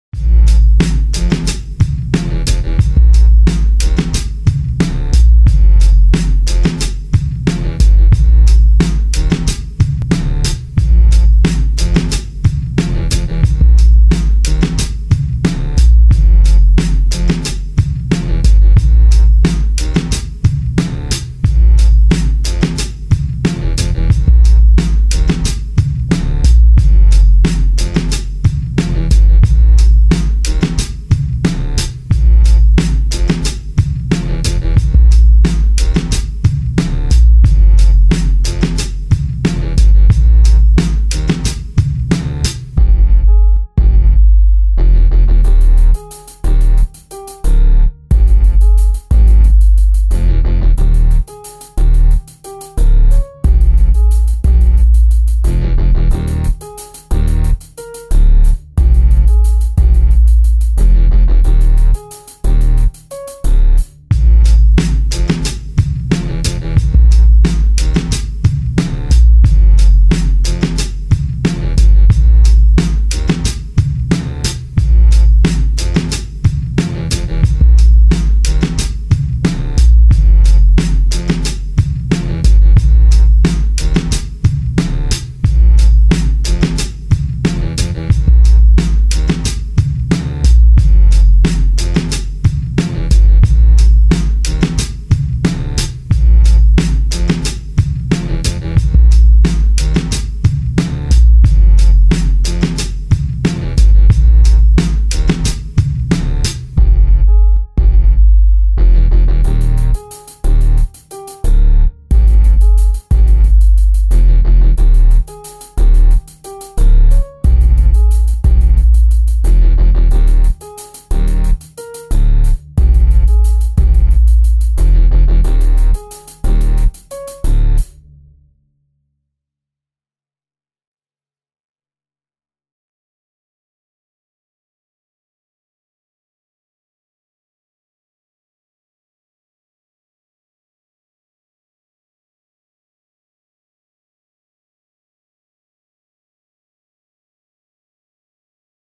basehead instrumental, logic 9
I made this beat in logic 9, let me know if yall like it!